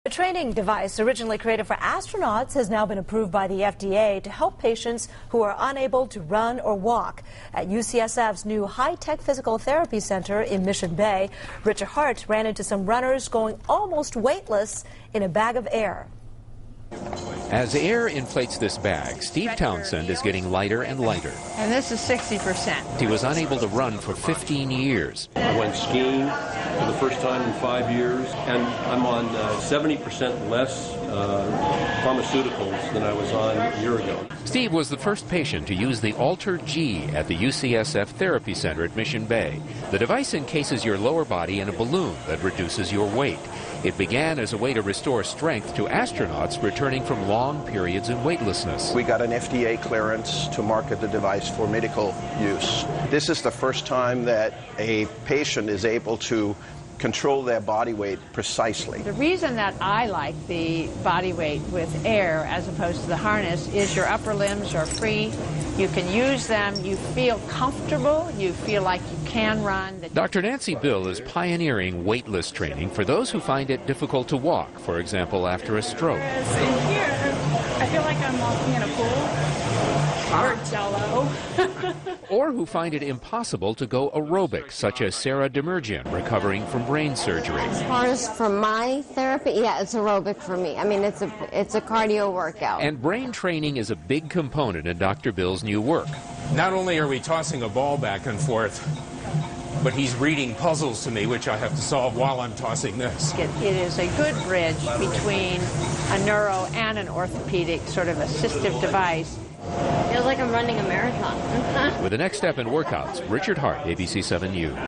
Pioneering Partial Weight Bearing Therapy
Watch how the AlterG is being used as an orthopedic and neurologic assistive device.  AlterG's Antigravity Treadmill is ideal for cardiac rehabilitation and physical therapy exercise.